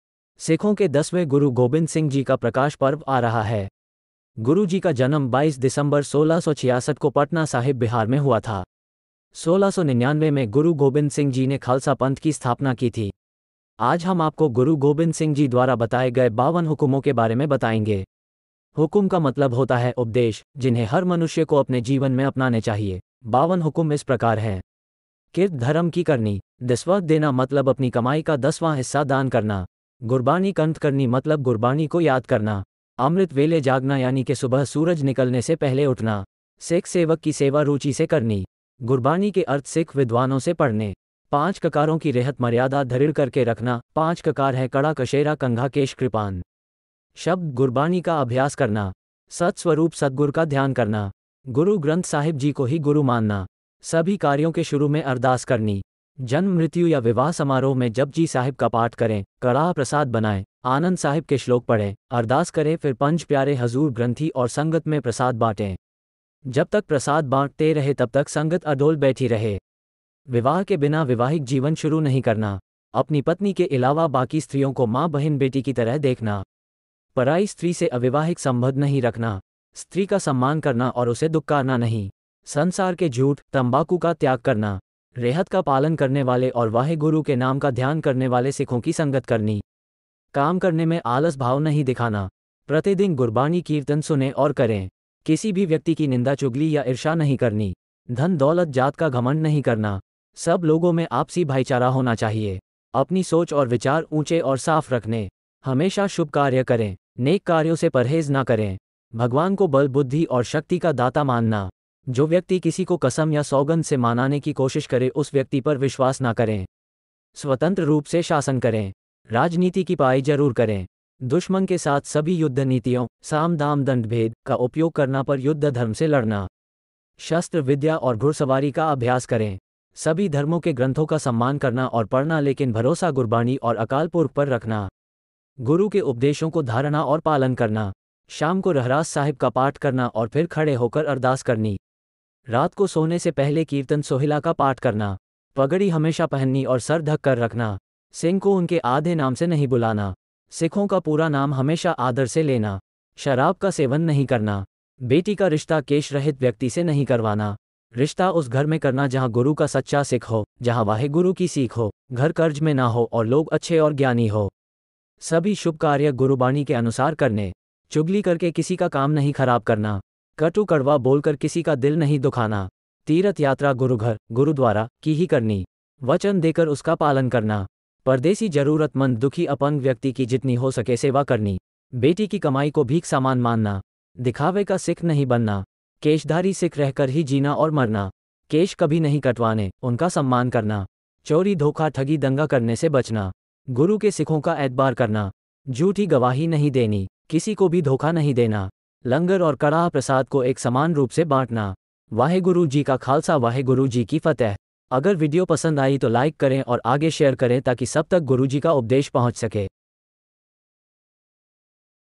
Guru Gobind Singh Ji ke 52 Hukum गुरु गोबिंद सिंह जी के 52 hukum हिन्दी में Sikh Guru 2023
Guru Gobind Singh Ji ke 52 Hukum

सिखों के दसवें गुरु गोबिंद सिंह जी का प्रकाश पर्व आ रहा है ! गुरु जी का जनम २२ दिसंबर १६६६ को पटना साहिब बिहार में  हुआ था ! १६९९ में गुरु गोबिंद सिंह जी ने खालसा पंथ की स्थापना की थी ! आज हम आपको गुरु गोबिंद सिंह जी द्वारा बताये गए ५२ हुकुमो के बारे में बताएँगे ! हुकुम का मतलब होता है उपदेश , जिन्हे हर मनुष्य को अपने जीवन में अपनाने  चाहिए !५२ हुकुम इस प्रकार हैं /
किरत  धरम की   करनी
दसवंद  देना  मतलब अपनी कमाई का दसवां हिस्सा दान करना
गुरबाणी  कंठ  करनी मतलब गुरबाणी को याद करना
अमृत वेले जागना यानि के सुबह सूरज निकलने से पहले उठना
सिख सेवक की सेवा रूचि से करनी
गुरबाणी के अर्थ सिख विद्वानों से पढ़ने
पांच ककारों की रेहत मर्यादा ढृढ़ करके रखना  ( पांच ककार हैं कड़ा कशेहरा  कंघा केश  कृपान )
शब्द गुरबाणी का अभ्यास करना
सत स्वरुप  सतगुर  का  ध्यान  करना
गुरु  ग्रन्थ  साहिब  जी को ही  गुरु  मानना
सभी कार्यों के शुरू में  अरदास करनी
जन्म मृत्यु या विवाह समारोह  में जपजी साहिब का पाठ करें , कड़ाह प्रसाद बनाएं , आनंद साहिब के श्लोक पढ़ें , अरदास करें  फिर पंज प्यारे हज़ूर   ग्रंथि  और संगत में प्रसाद बांटें
जब तक प्रसाद बाँट ते रहे तब तक संगत अडोल बैठी रहे
विवाह के बिना विवाहिक जीवन शुरू नहीं करना
अपनी पत्नी के इलावा बाकी स्त्रियों  को माँ बहिन बेटी की तरह देखना
परायी स्त्री से   अविवाहिक  सम्भन्द नहीं रखना
स्त्री का सम्मान करना और उसे दुक्कारना  नहीं
संसार के झूठ , तम्बाकू का त्याग   करना
रेहत का पालन करने वाले और वाहेगुरु के नाम का ध्यान करने वाले सिखों की संगत करनी
काम करने में  आलस भाव नहीं दिखाना
प्रतिदिन गुरबाणी कीर्तन सुने और करें
किसी भी व्यक्ति की निंदा चुगली या इर्षा नहीं करनी
धन दौलत जात का घमंड नहीं करना ! सब  लोगो में आपसी भाईचारा होना चाहिए
अपनी सोच और विचार ऊंचे और साफ़ रखने
हमेशा शुभ कार्य करें ! नेक कार्यों से परहेज़ ना करें
भगवान को बल बुद्धि और शक्ति का दाता मान ना
जो व्यक्ति किसी को कसम या सौगंध से मानाने की कोशिश करे उस व्यक्ति पर विशवास ना करें
स्वतंत्र रूप से शासन करें
राजनीती की पढाई जरूर करें
दुश्मन के साथ सभी युद्ध नीतियों ( साम दाम दंड भेद ) का उपयोग करना पर युद्ध धर्म से लड़ना
शस्त्र विद्या और घुड़सवारी का अभ्यास करें
सभी  धर्मोँ के ग्रंथो का सम्मान करना और पढ़ना लेकिन भरोसा गुरबाणी और अकाल पुरख पर रखना
गुरु के उपदेशों को धारणा  और पालन करना
शाम को रहरास साहिब का पाठ करना और फिर खड़े होकर अरदास करनी
रात को सोने से पहले कीर्तन सोहिला का पाठ करना
पगड़ी हमेशा पहननी और सर ढक कर रखना
सिंह को उनके आधे नाम से नहीं बुलाना ! सिखों का पूरा नाम हमेशा आदर से लेना
शराब का सेवन नहीं करना
बेटी का रिश्ता केश रहित व्यक्ति  से नहीं करवाना ! रिश्ता उस घर में करना जहां गुरु का सच्चा सिख  हो , जहां वाहेगुरु  की सीख हो।  घर कर्ज में ना हो  और लोग अच्छे और ज्ञानी हो
सभी शुभ कार्य गुरुबाणी के अनुसार करने
चुगली करके किसी का काम नहीं खराब करना
कटु कड़वा बोलकर किसी का दिल नहीं दुखाना
तीर्थ यात्रा गुरुघर ( गुरुद्वारा ) की ही करनी
वचन देकर उसका पालन करना
परदेसी जरूरतमंद दुखी अपंग व्यक्ति की जितनी हो सके सेवा करनी
बेटी की कमाई को भीख सामान मानना
दिखावे का सिख नहीं बनना
केशधारी सिख रहकर ही जीना और मरना ! केश कभी नहीं कटवाने , उनका सम्मान करना
चोरी धोखा  ठगी  दंगा करने  से बचना
गुरु के सिखों का एतबार करना
झूठी गवाही नहीं देनी
किसी को भी धोखा नहीं देना
लंगर और कड़ाह प्रसाद को एक समान रूप से बांटना
वाहेगुरु जी का खालसा वाहेगुरु जी की फ़तेह